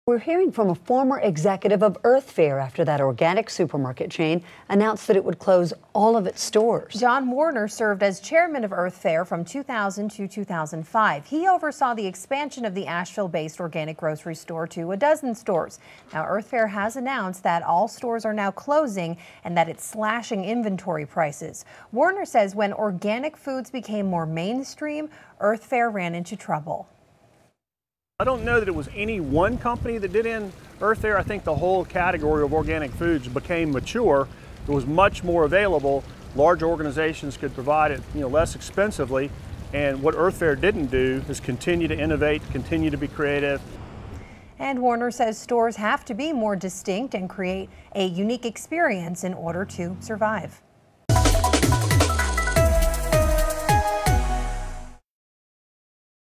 Asheville-based Earth Fare to close all stores
The Asheville-based natural and organic grocery store and supermarket Earth Fare announced Monday that it will close all of its stores and begin inventory liquidation sales at all outlets.